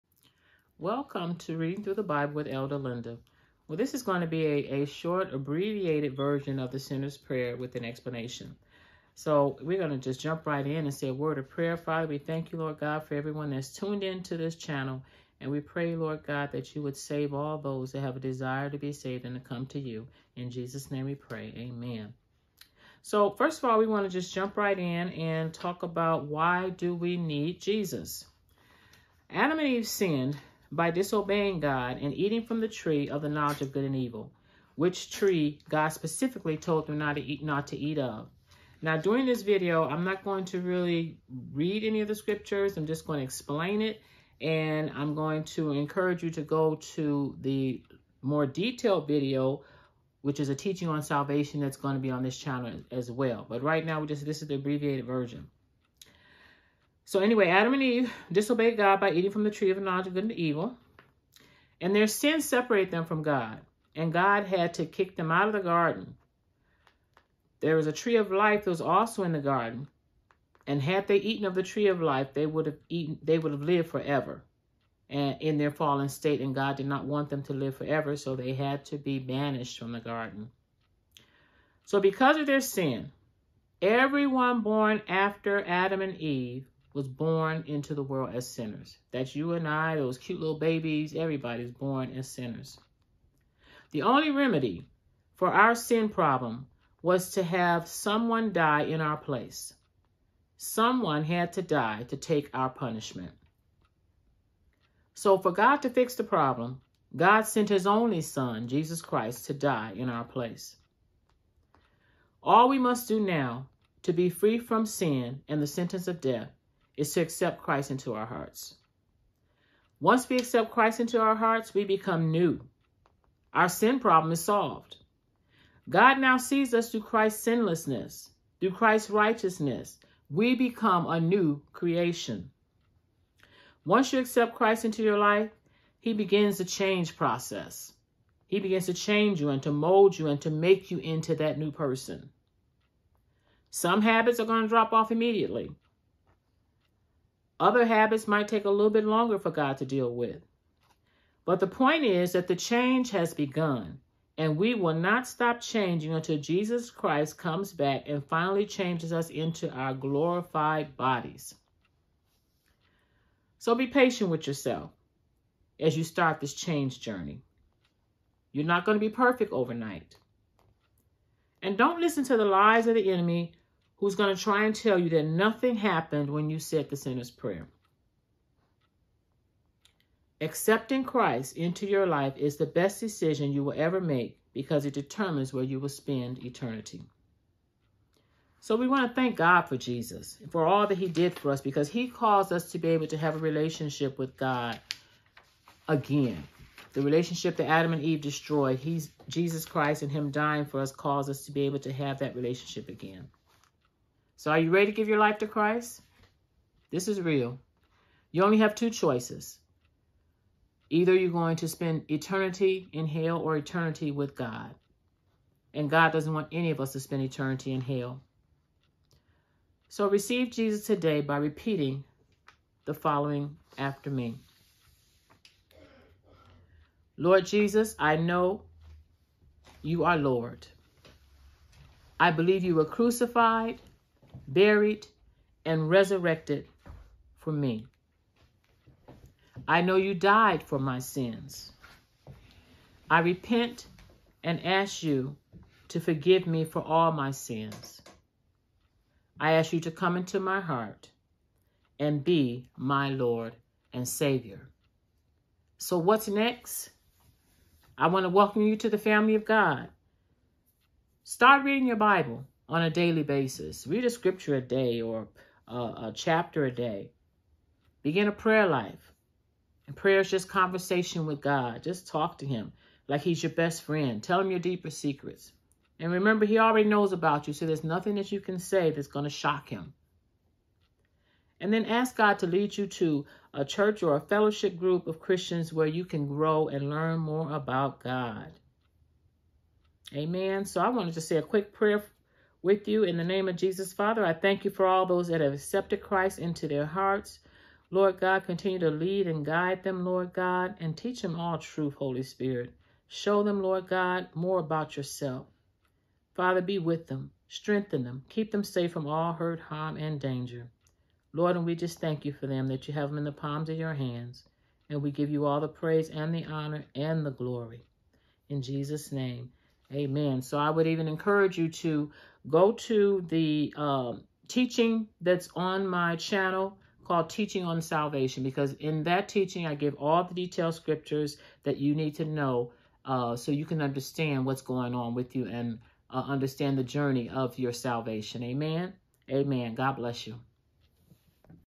Sinners Prayer (short version)
Short explanation of why we need salvation. Sinners prayer included. (For a more in-depth study on salvation see video called ‘Teaching on Salvation’.)